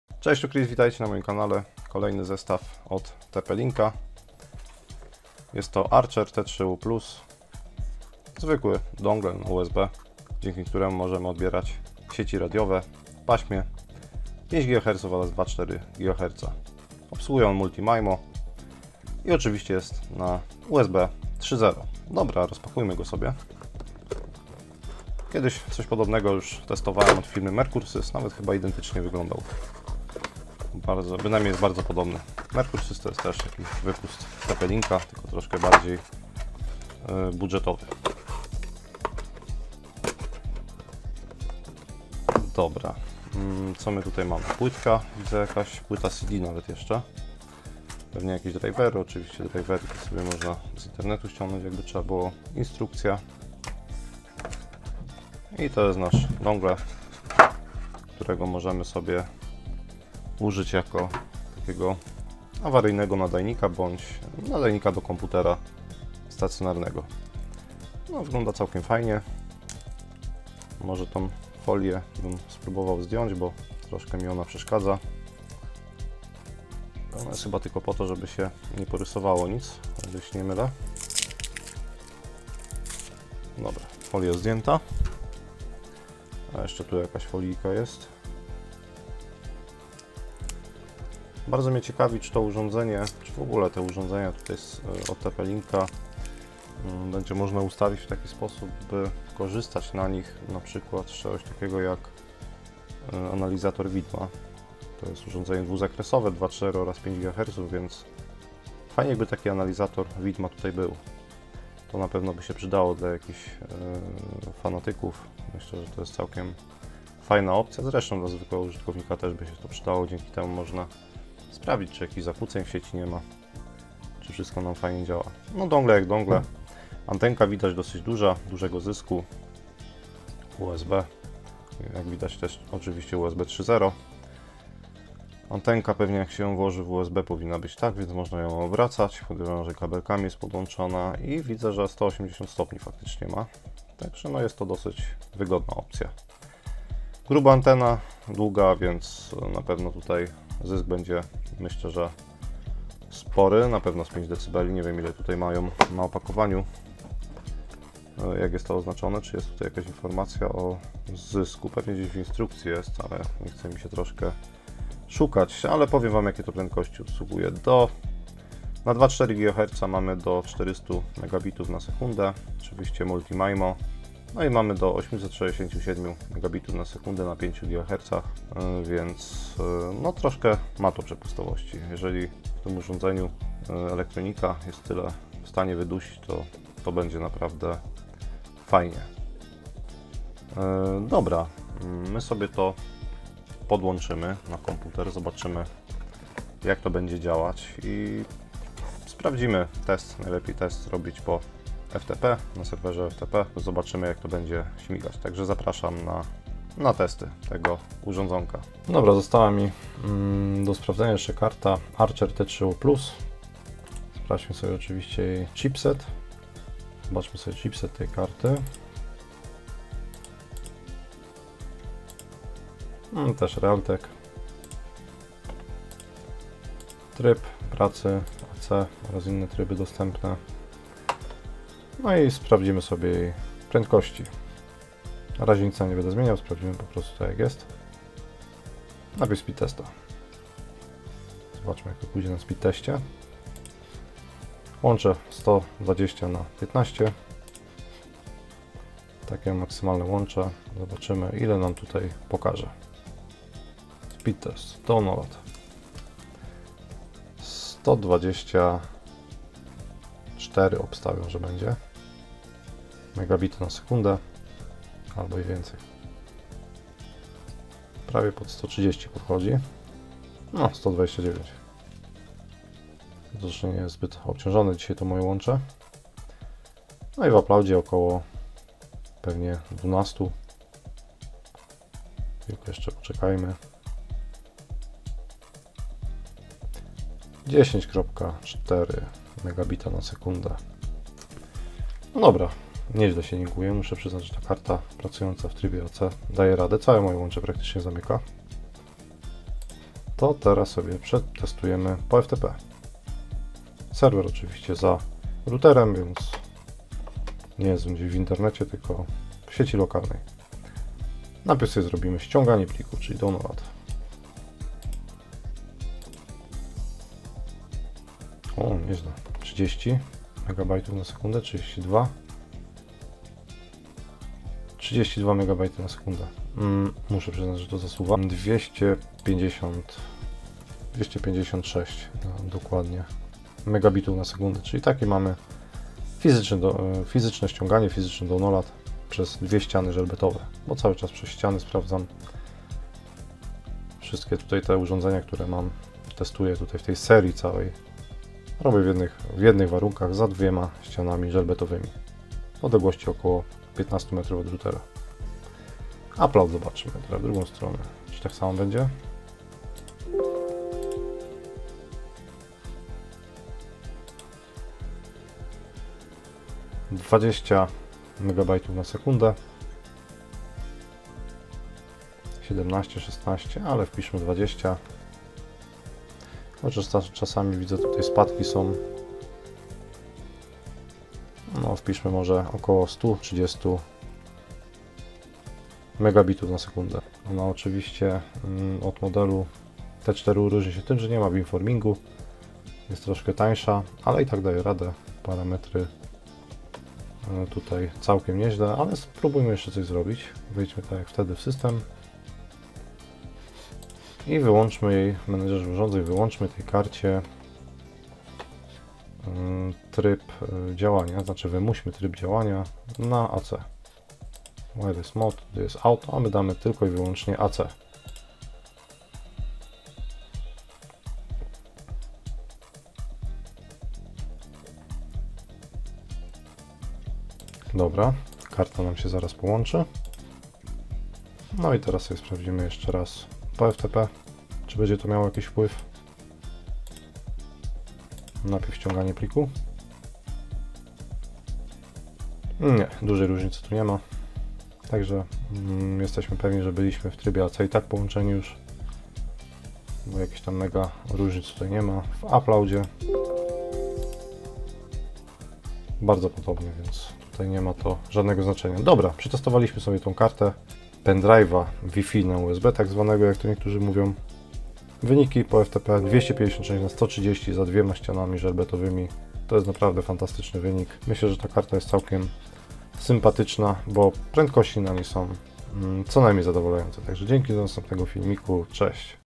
TP-LINK - ARCHER T3U PLUS - Pendrive WiFi 2.4 oraz 5Ghz na USB 3.0 MIMO 2x2
Ciekawa i tania karta sieciowa na USB od TP-LINKa Jeśli chcesz się ze mną skontaktować: